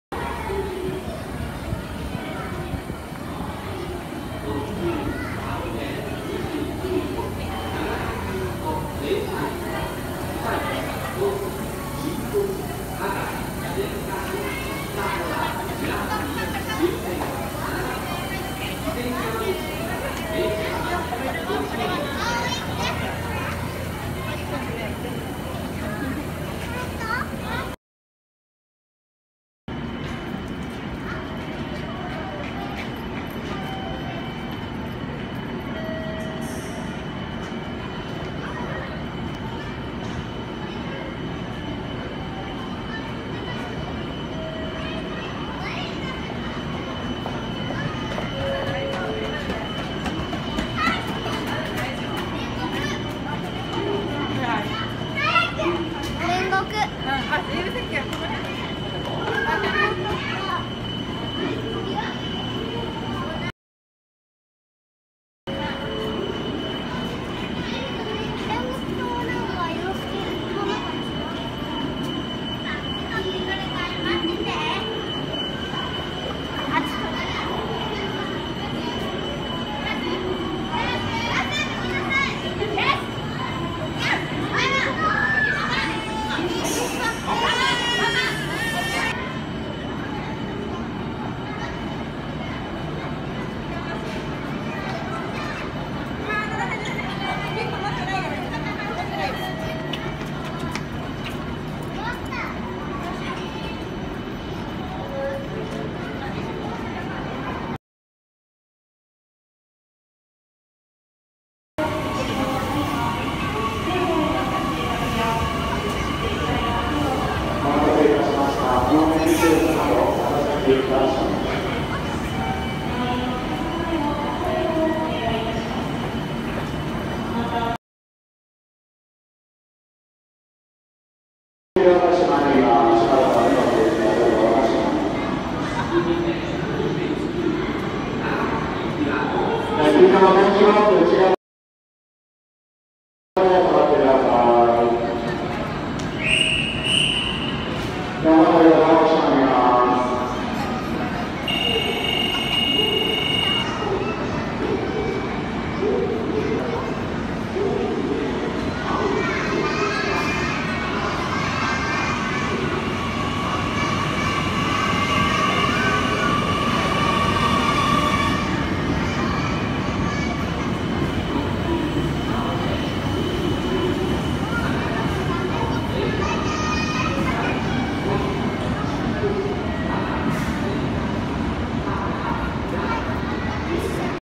【鬼滅の刃】JR九州☆ラッピング列車 (その2)
JR九州の鬼滅の刃ラッピング列車を撮影してきました。
とってもカッコいい！